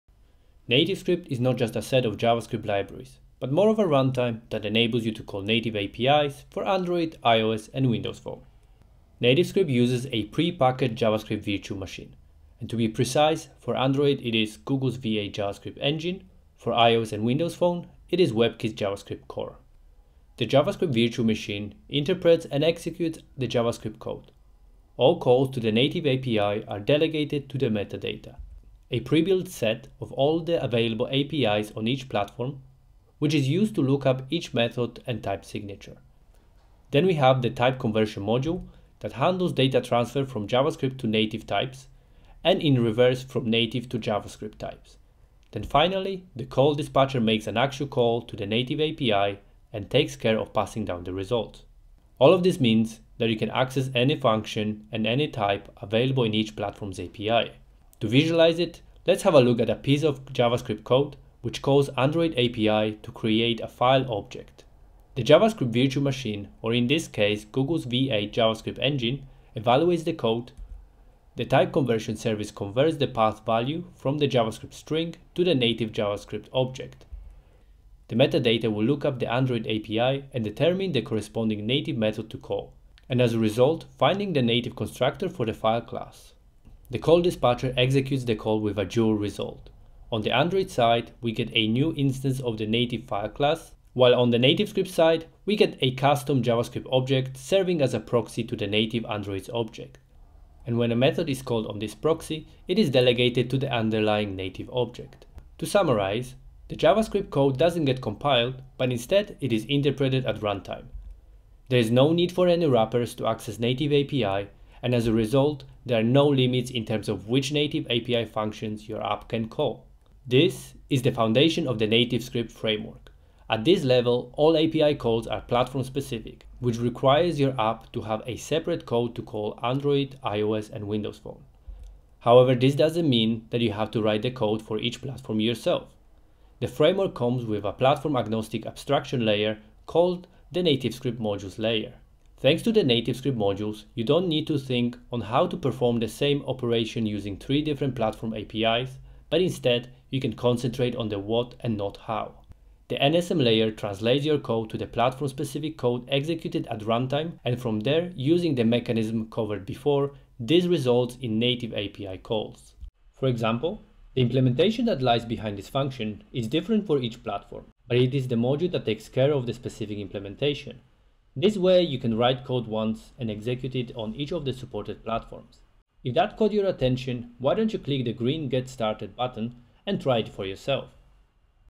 How NativeScript Works
NativeScript enables developers to build native apps for iOS, Android and Windows Universal while sharing the application code across the platforms. When building the application UI, developers use our libraries, which abstract the differences between the native platforms.